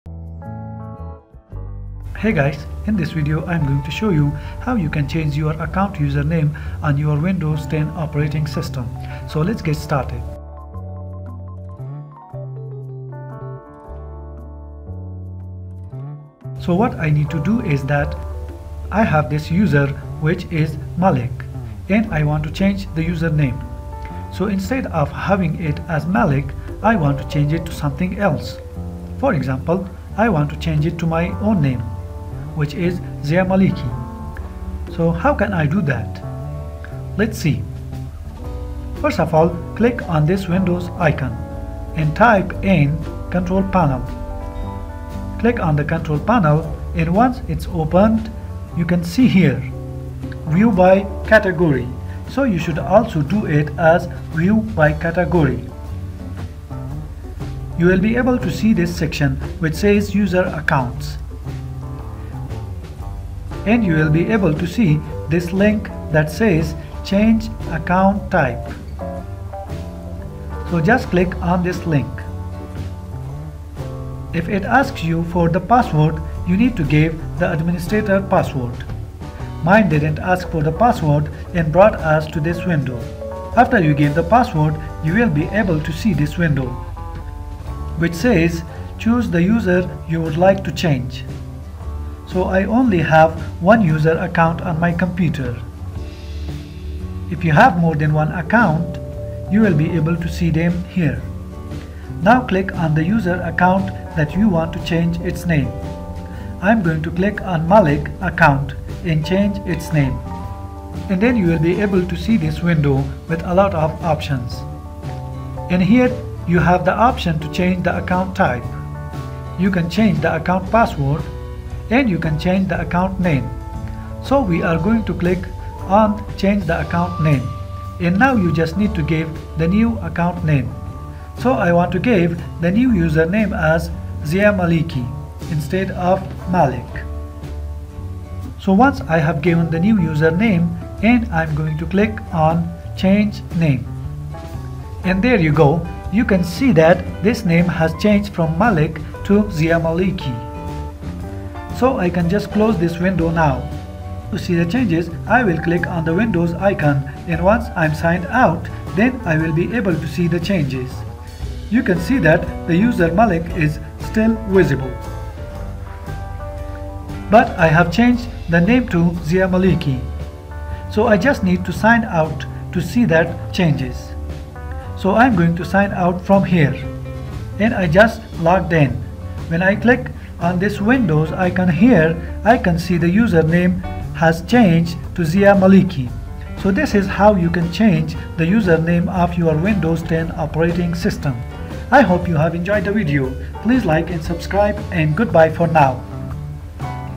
how to change user name of account in windows 10
In this short tutorial I am going to show you, how you can change your account name in windows 10 operating system. It's a very easy tutorial and you just need to follow these simple steps.